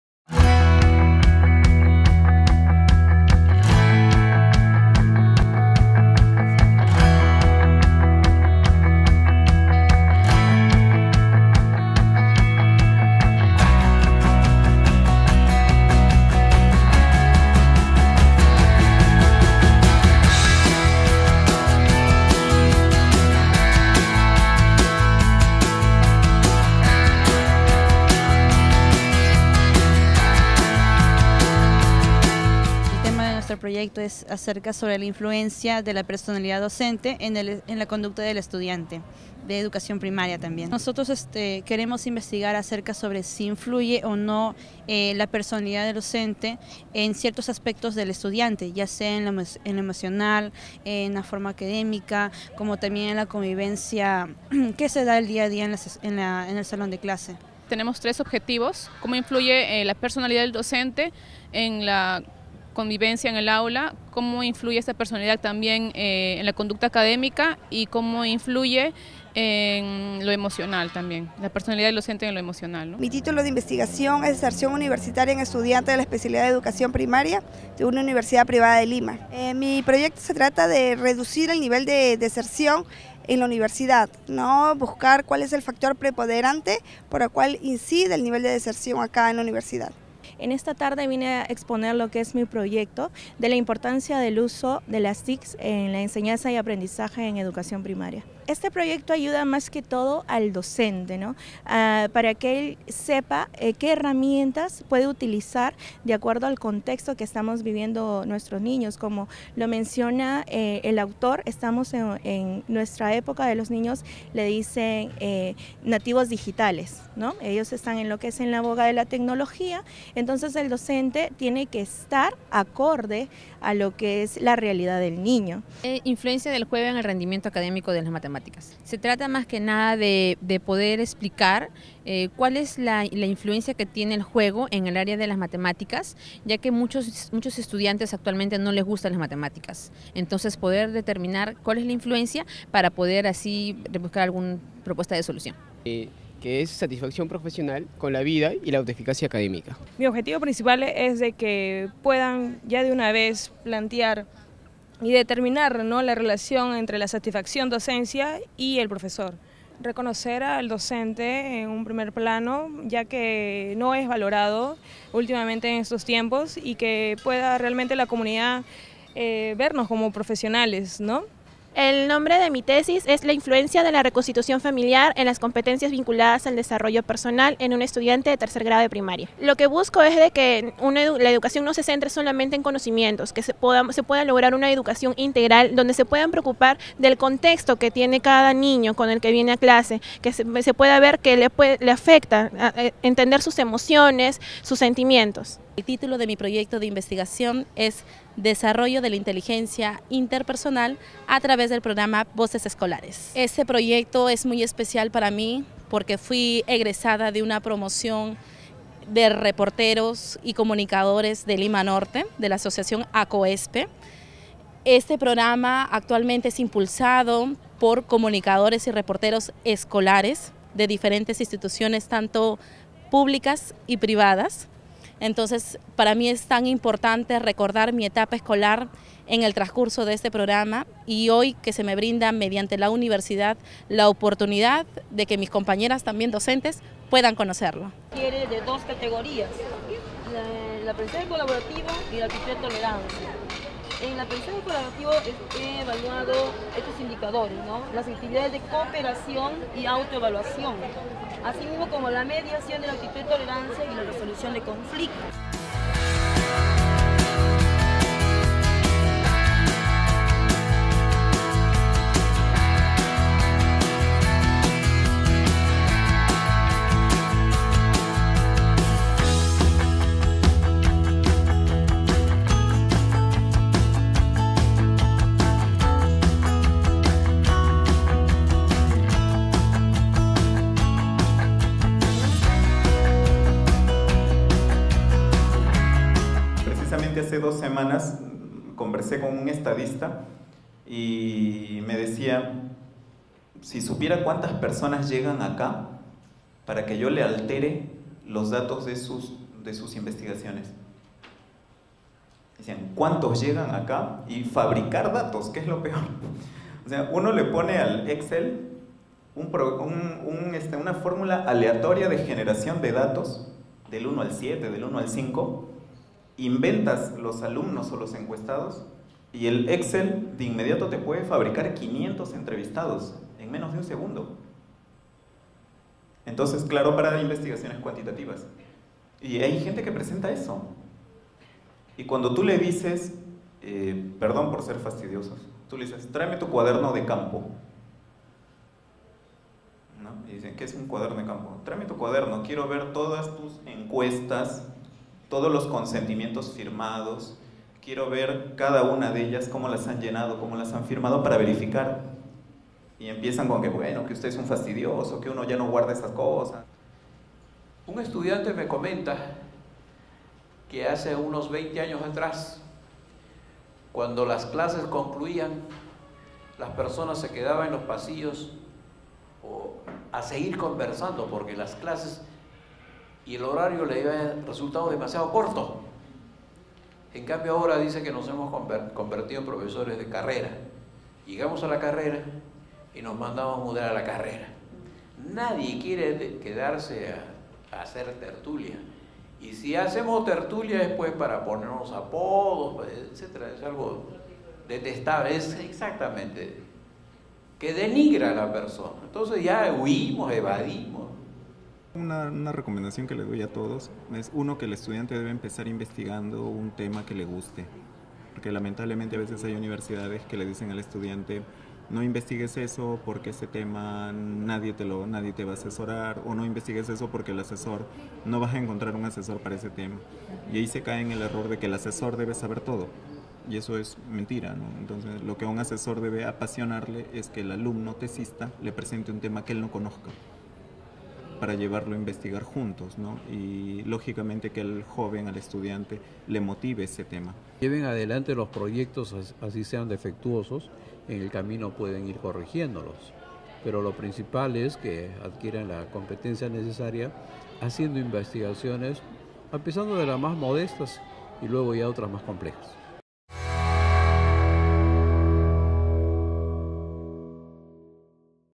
II Congreso de Investigación UCH_ EDUCACIÓN PRIMARIA
La Facultad de Educación Primaria e Interculturalidad inició la jornada del II Congreso de Investigación de Ciencias y Humanidades el lunes 20 de noviembre.